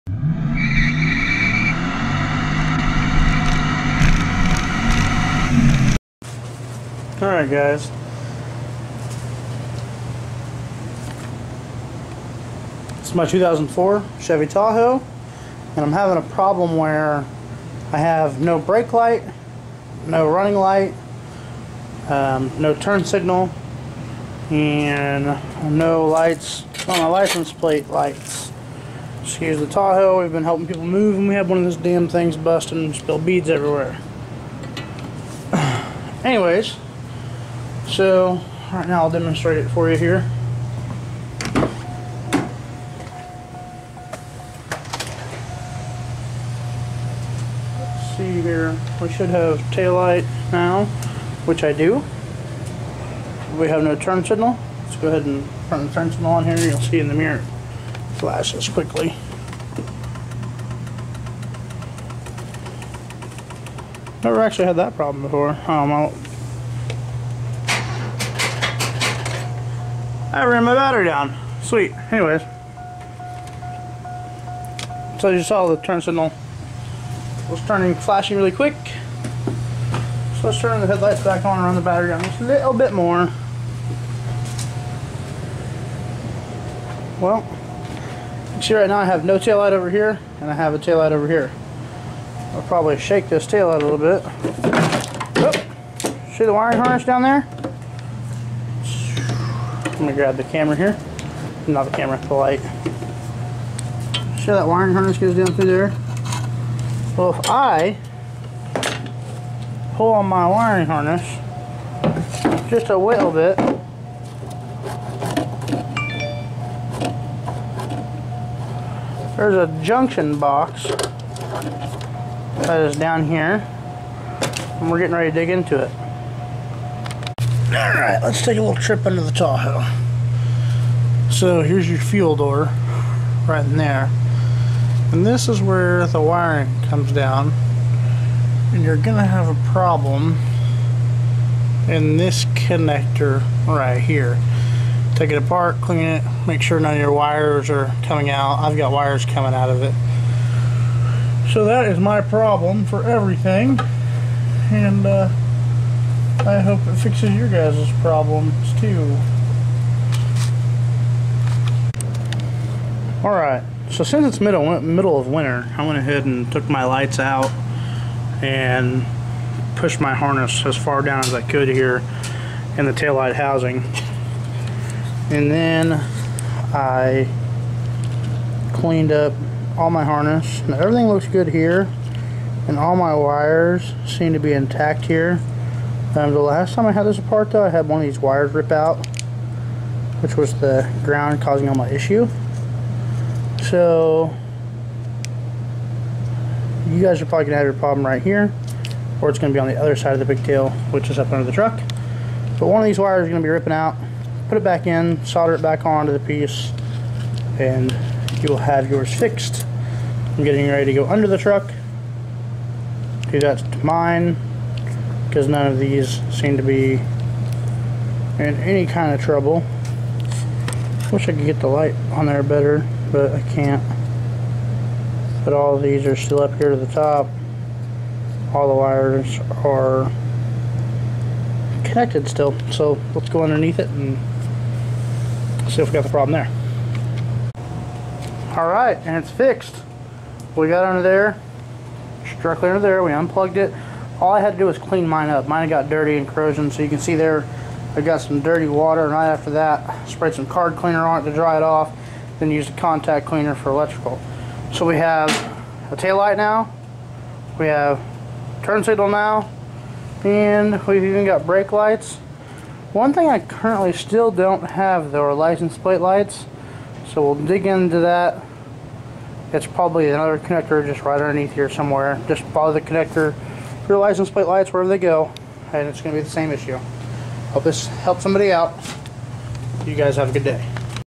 2004 Chevy Tahoe How to fix no tail light, or no turn signal, or no brake lights or all 3
In this Video I explain why the rear lights might not work on your Tahoe, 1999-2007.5 Tahoe, Yukon, suburban, Denali, and others should be the same. Turn signal, lights.